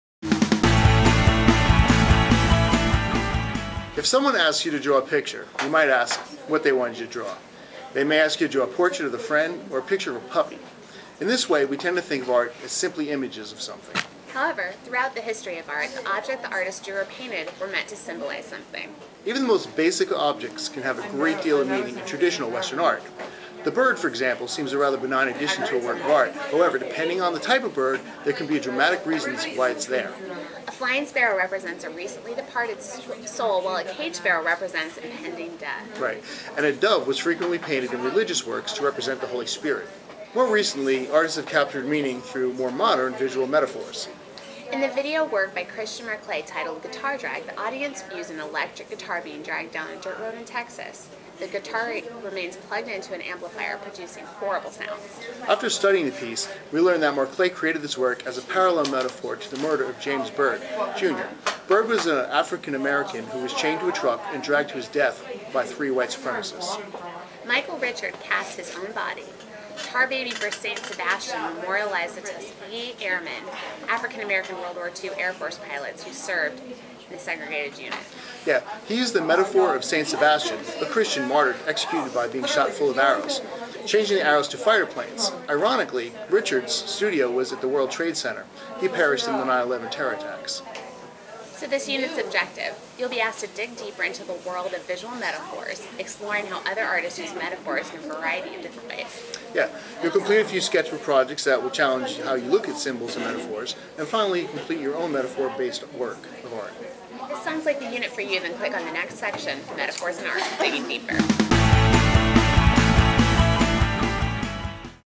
Metaphor in Art : Introduction
In this unit’s objective you’ll be asked to dig deeper into the world of visual metaphors art, exploring how other artists use metaphors in a variety of different ways. You’ll complete a few sketchbook projects that will challenge how you look at symbols and metaphors. Finally, you’ll complete your own metaphor based art work.